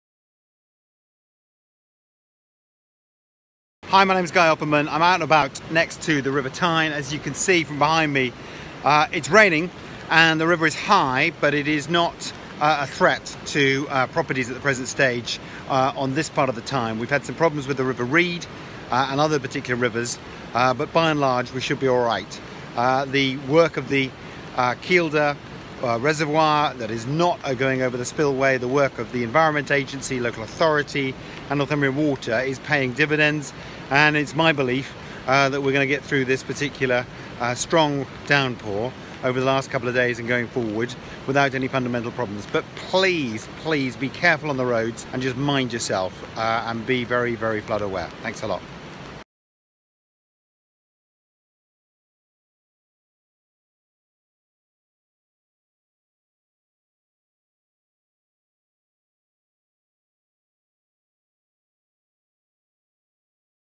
Flood Risk Update from Guy Opperman MP
An update on local flood risk from Tynedale and Ponteland's local MP Guy Opperman.

With significant rainfall across Northumberland, Guy has been visiting many of the villages affected by high river water levels
I have spoken with the Environment Agency and local Northumberland County Council Councillors for updates, and at the present stage it appears not to be a threat to property along the river Tyne.

Thanks to the work of Northumbrian Water, with reduced levels Kielder reservoir, water is not flooding over the spillway, which helps to reduce the risk of flooding further down the Tyne Valley.

There has been some problems with the River Rede, and Guy will be visiting myself tomorrow to see the problems for himself.

If you need to report a flooding problem, you can contact the Environment Agency Floodline 24 hours a day on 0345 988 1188.